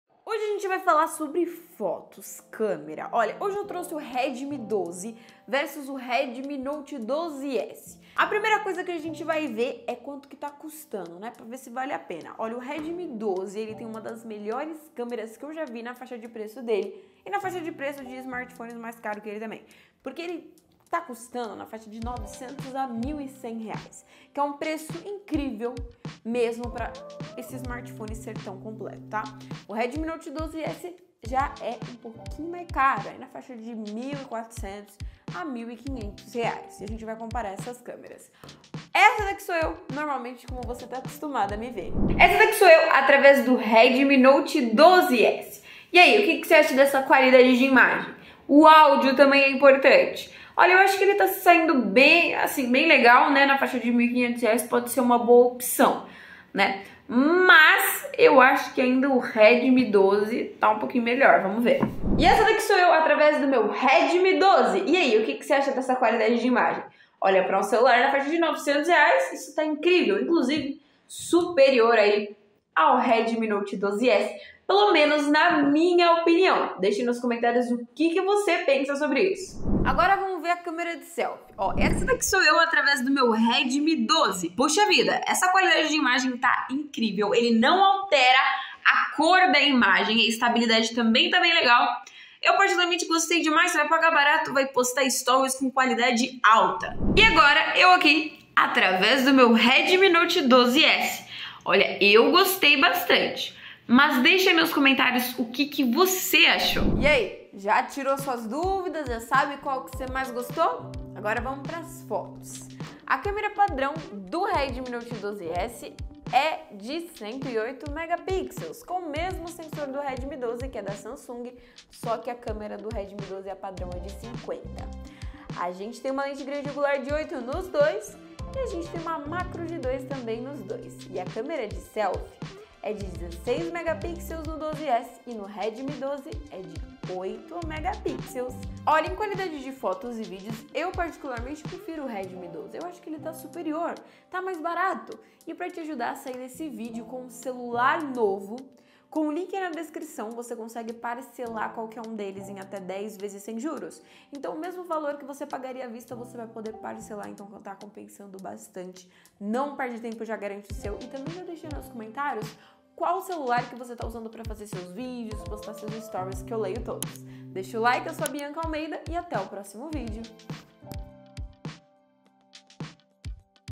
Redmi Note 12s vs Redmi 12 qual tem as MELHORES CAMERAS? Falei a Verdade - comparativo
✅Redmi 12
Outras CORES

Redmi note 12 (Promoção)
Xiaomi POCO X5 PRO
iPhone 11 (Mais vendido)